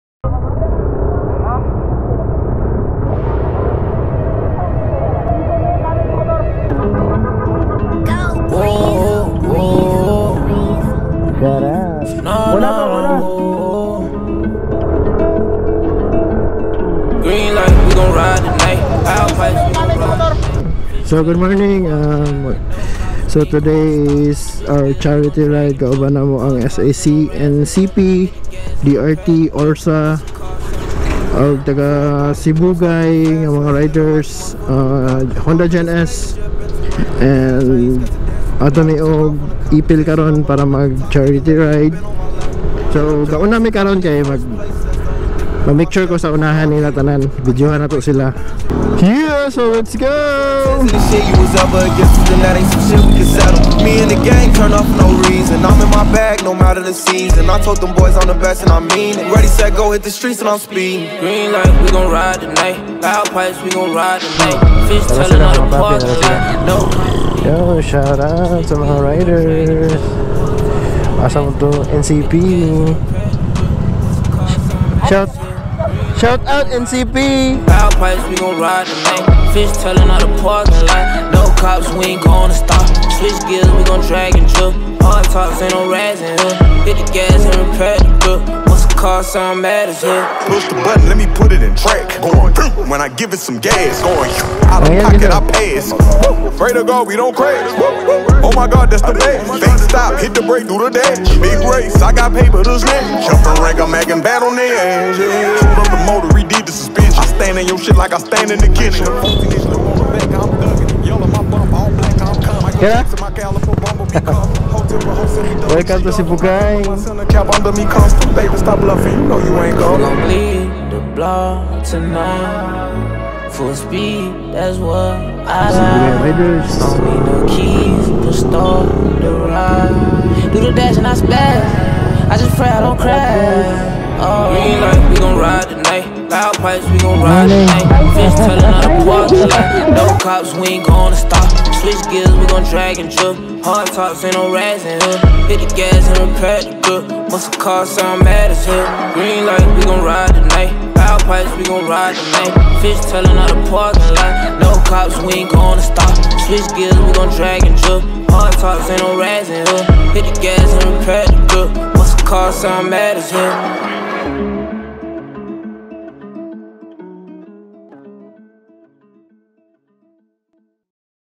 RIDE FOR A CAUSE - IPIL, ZAMBOANGA SIBUGAY
RIDE FOR A CAUSE from Liloy, Zamboanga del Norte to Ipil, Zamboanga Sibugay for the benefit of PRINCESS ARIELLA GREGORI, a 4 years old with Cerebral Palsy & Pneumonia.